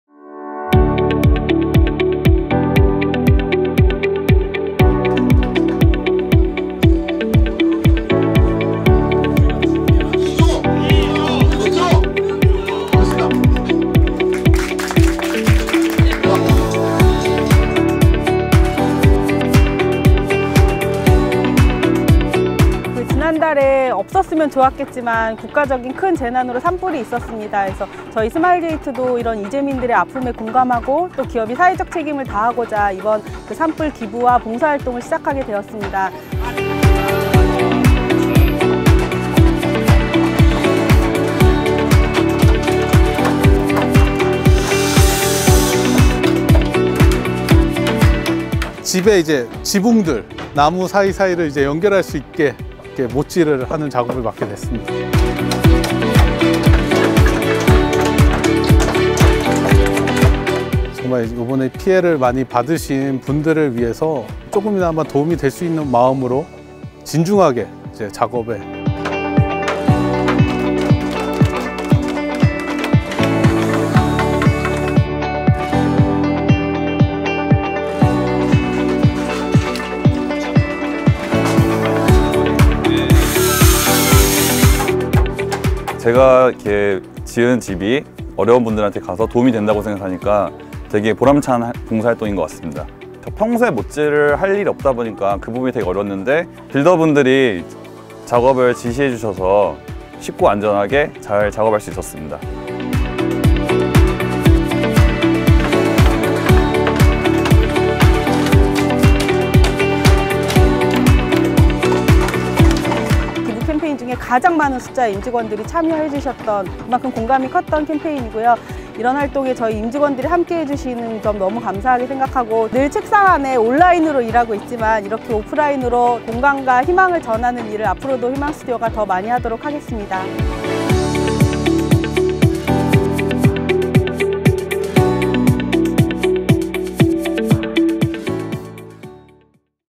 강원도 산불피해 주민들을 위해 집 지으러 가요!
스마일게이트 임직원들은 강원도 산불 피해로 집을 잃고 실의에 빠진 분들이 임시로 거주하실 수 있는 집을 짓고 왔습니다. 작은 도움의 손길과 우리의 마음이 어려운 분들에게 위로가 되셨으면 합니다.